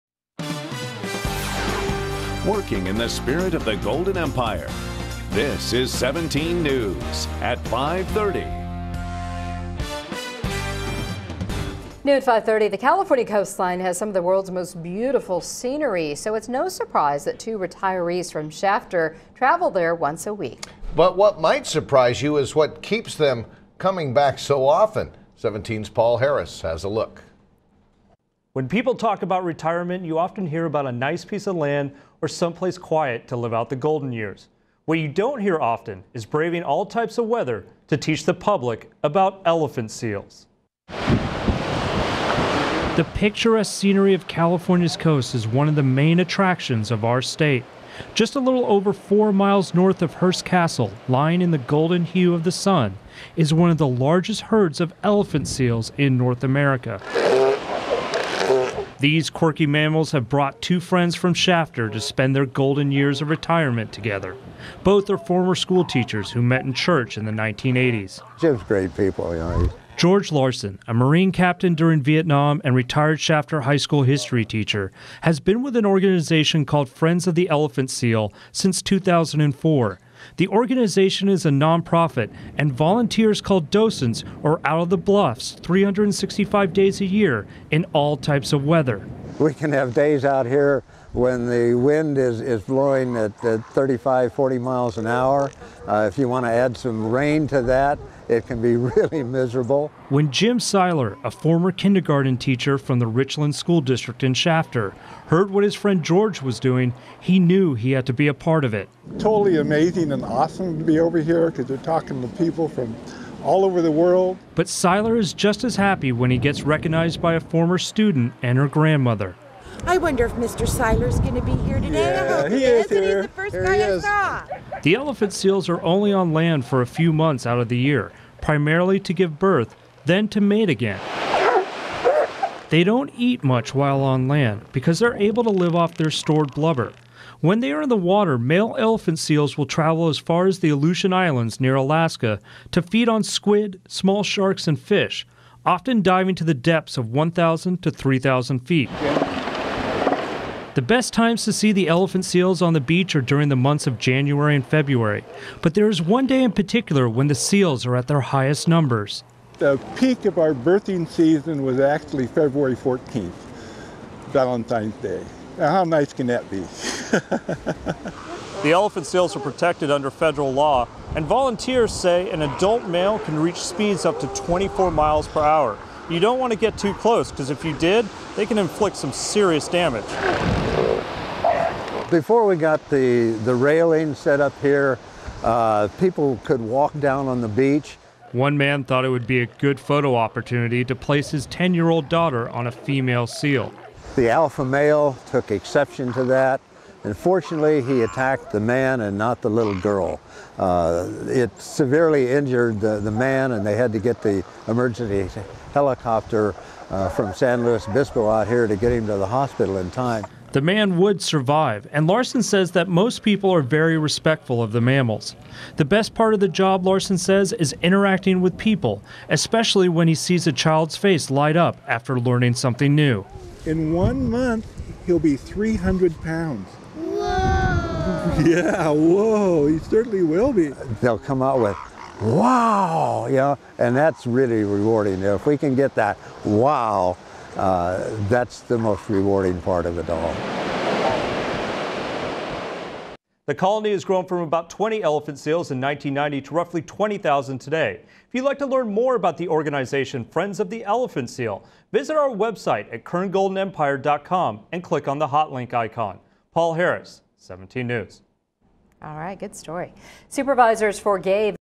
Friends of the Elephant Seal
A news story about friendship, the California coast and elephant seals.